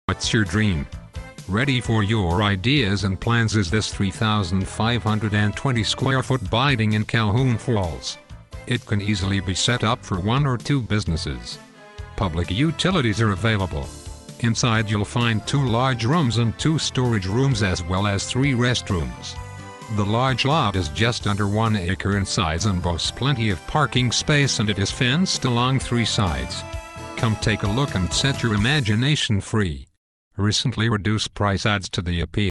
117 S Calhoun St Calhoun Falls SC
The only thing stopping you is your imagination! This concrete block building has a slab foundation and  3 restrooms  Located on in town lot with plenty of parking

Video uploaded on Wed 04 Nov, 2009